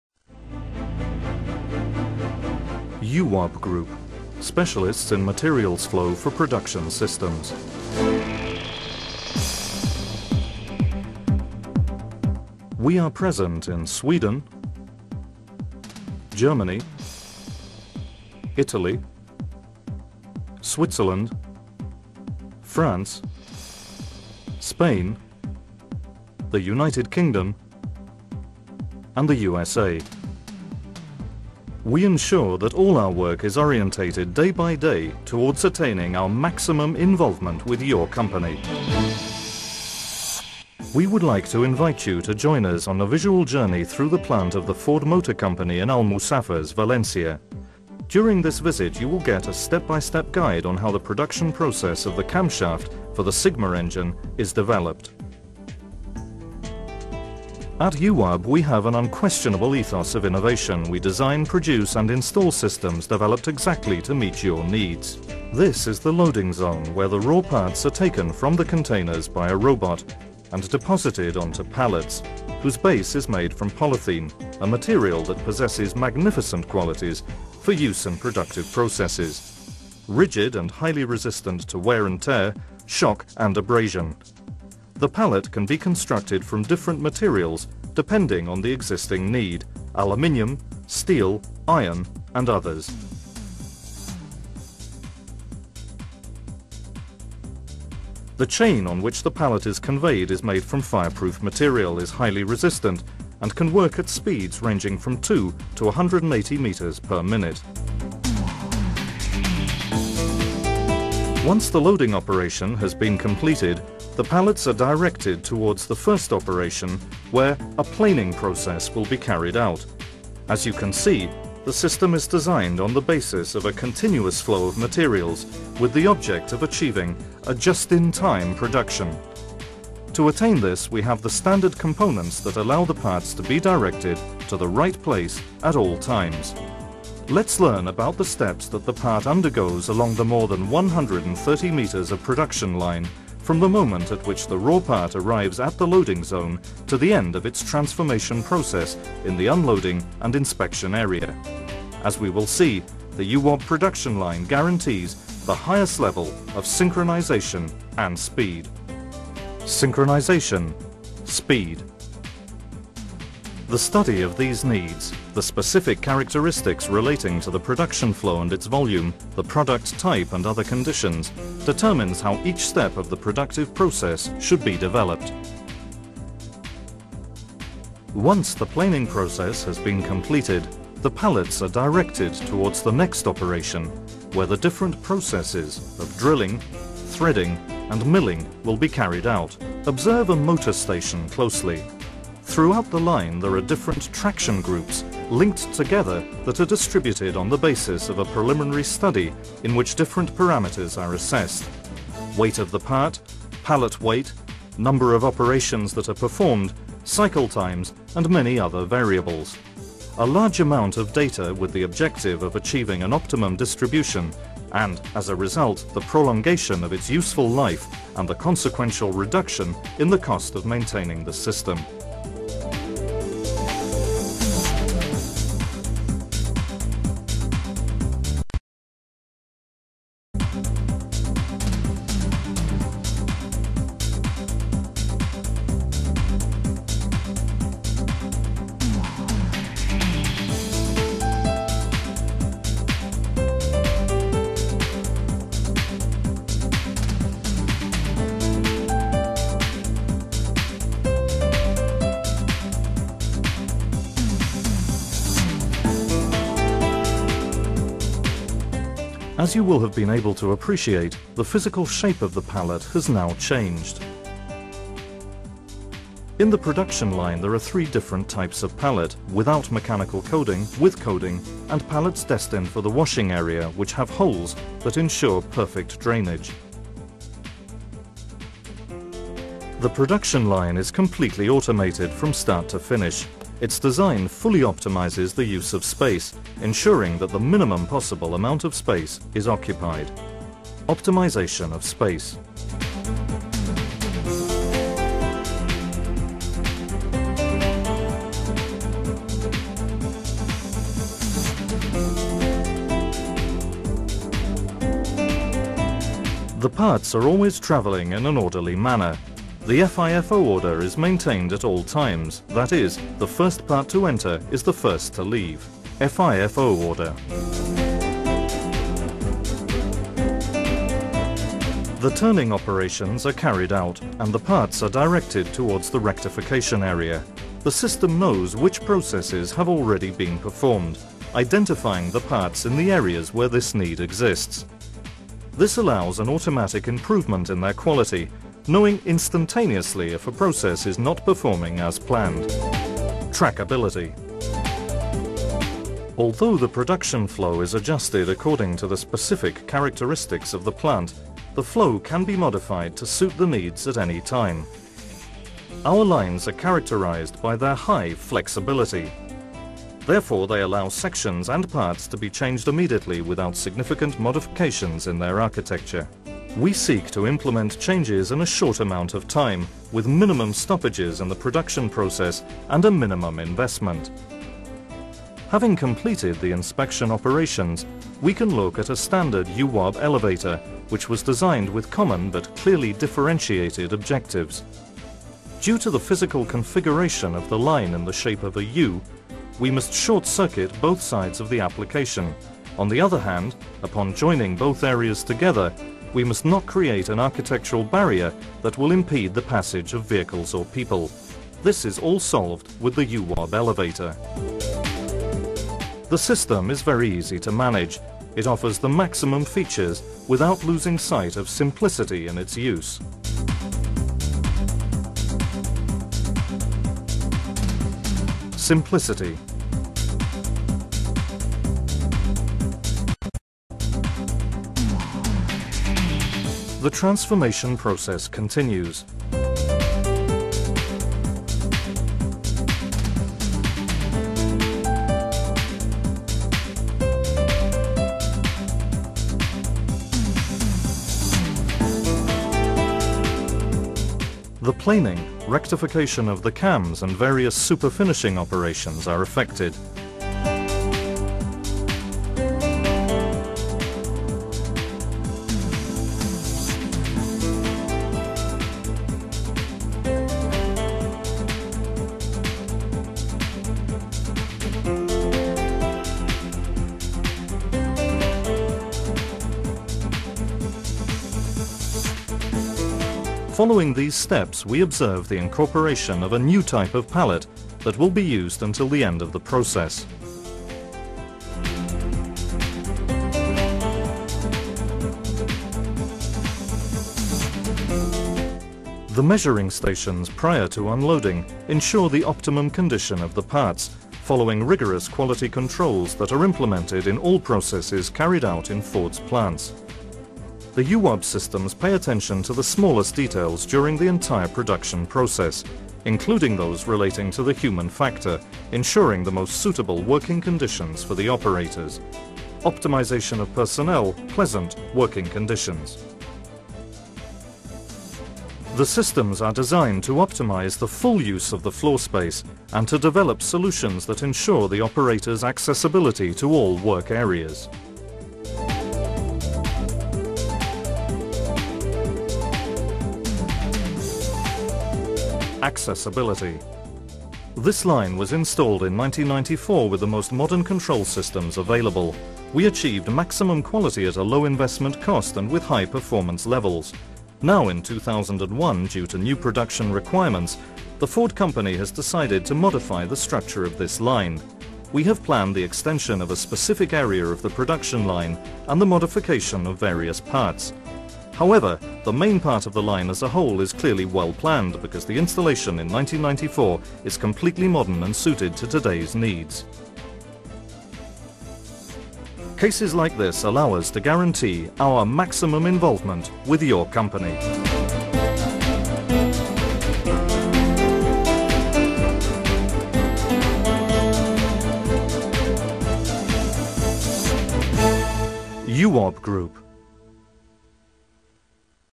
EWAB Engineering - EWAB Flow Technology for Lean Manufacturing and Assembly processes
EWAB Flow Technology for Lean Manufacturing and Assembly processes.

Workpiece Carrier System